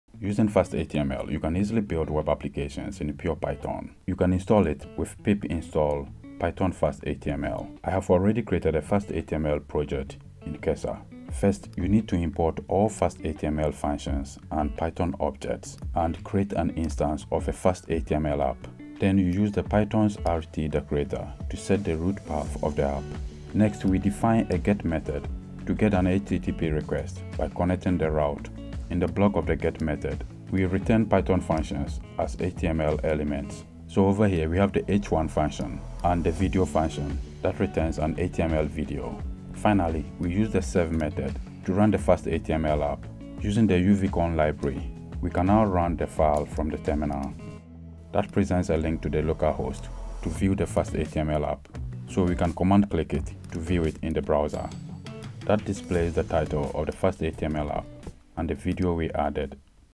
Python & FastHTML: Build a Simple Web App
This video shows you how to get started with FastHTML quickly to create modern web applications with a pure Python code. It is easy and fast to learn so give it a try if you do not want to learn JavaScript related tech like React or Next.js.